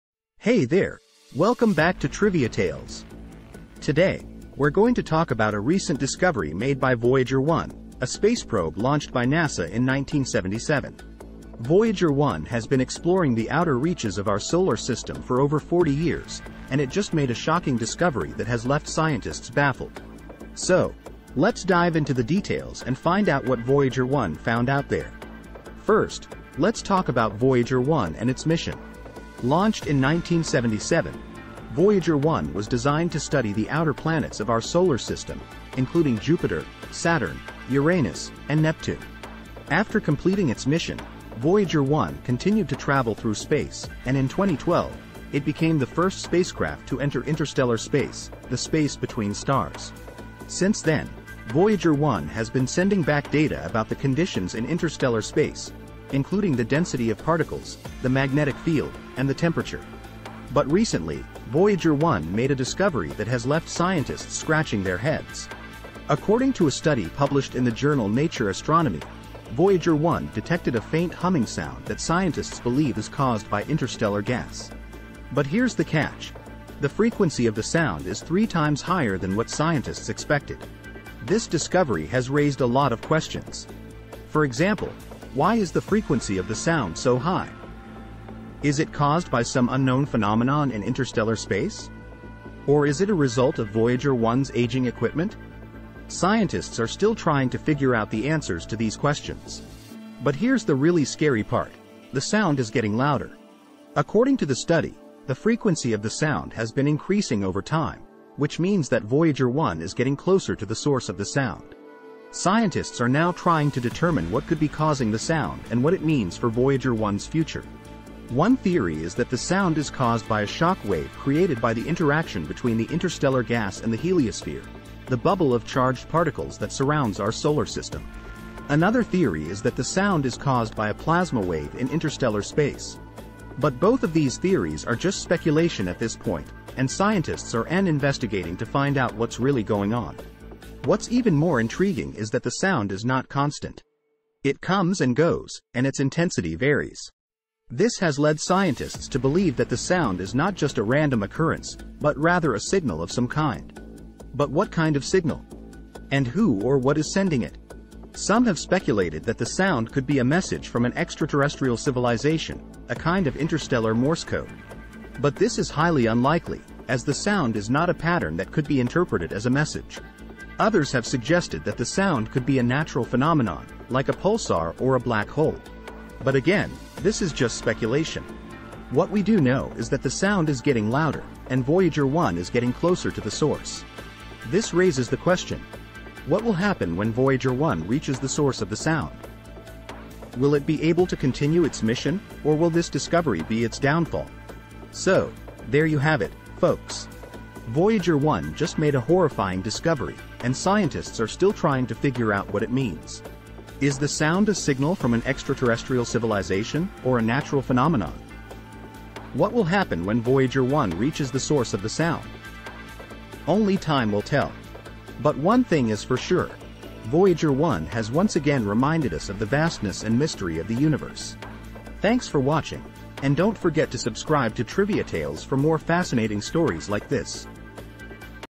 Voyager 1 Just Made A Horrifying Discovery
The script is for a video on the YouTube channel "Trivia Tales" and is titled "Voyager 1 Just Made A Horrifying Discovery". The video discusses a recent discovery made by Voyager 1, a space probe launched by NASA in 1977, which has been exploring the outer reaches of our solar system for over 40 years. The discovery is a faint humming sound that scientists believe is caused by interstellar gas, but the frequency of the sound is 3 times higher than what scientists expected. The video explores the theories behind the sound and what it could mean for Voyager 1's future. The video concludes by reminding viewers of the vastness and mystery of the universe.